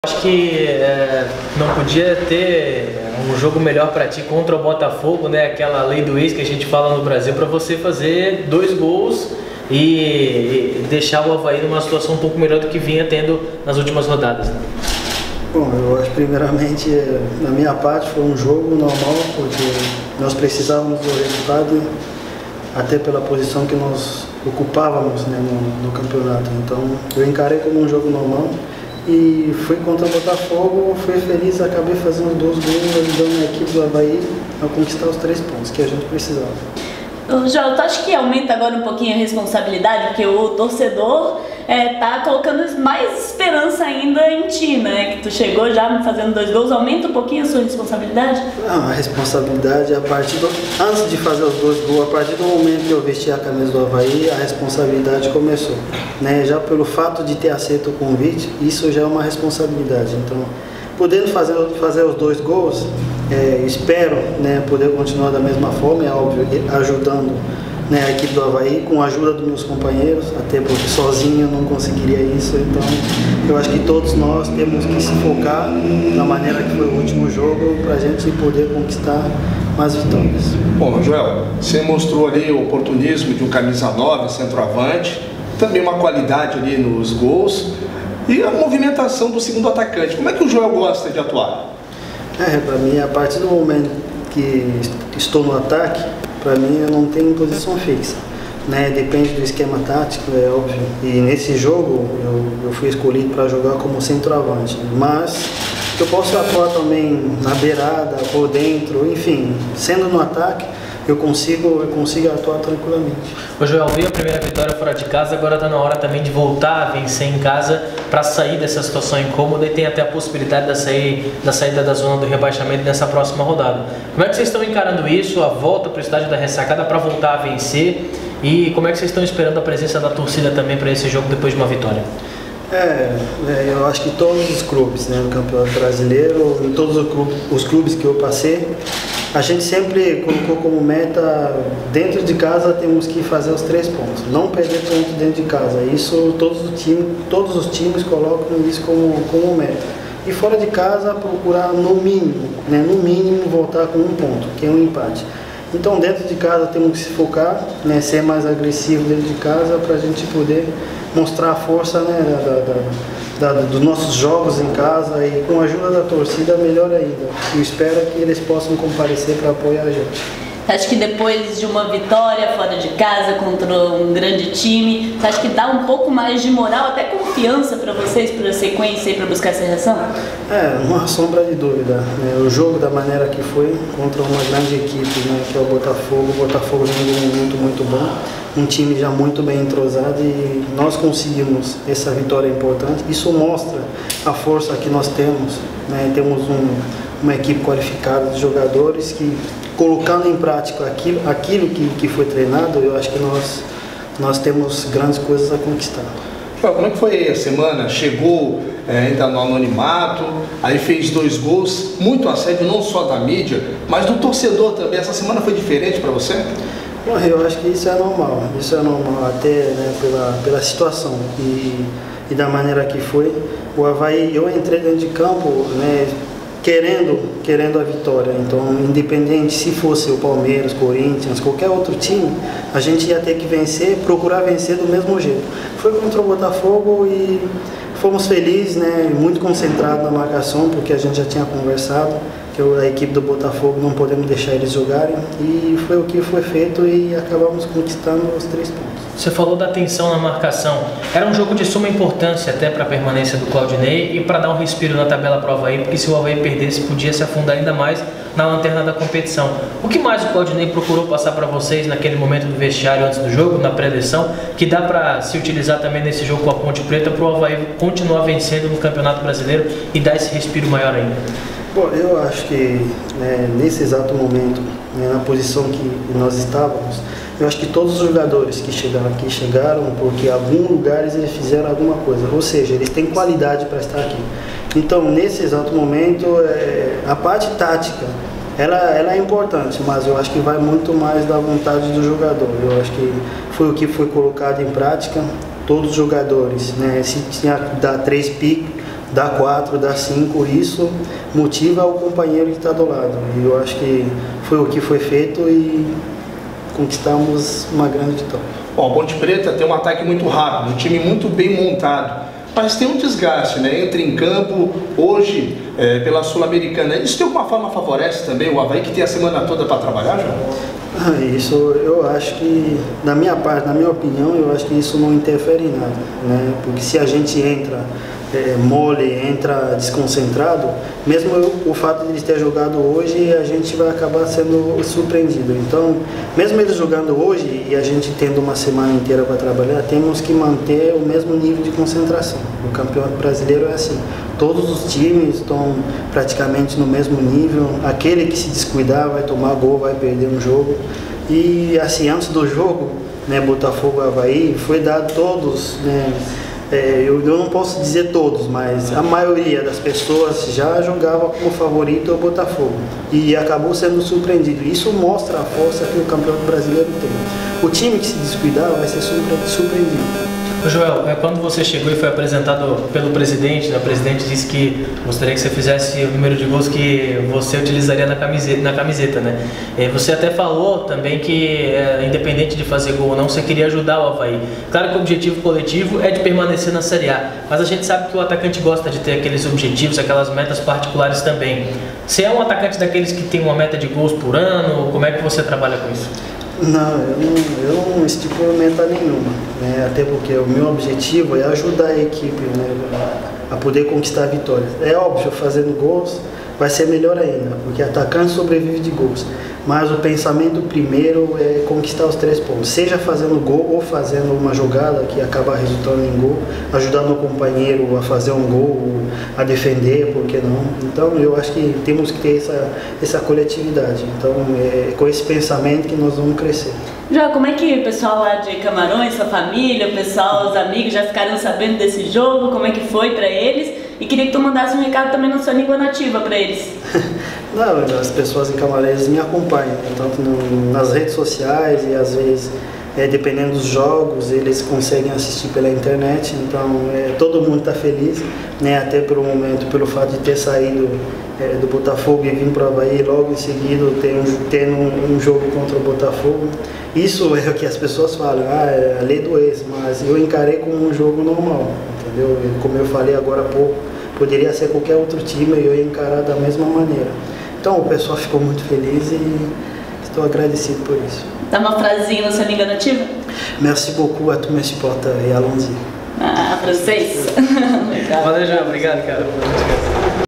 TV Avaí | COLETIVA | Joel - 29.06.2017
Confira a entrevista coletiva com o atacante Joel, ocorrida em 29 de junho de 2017, na Sala de Imprensa Tullo Cavallazzi.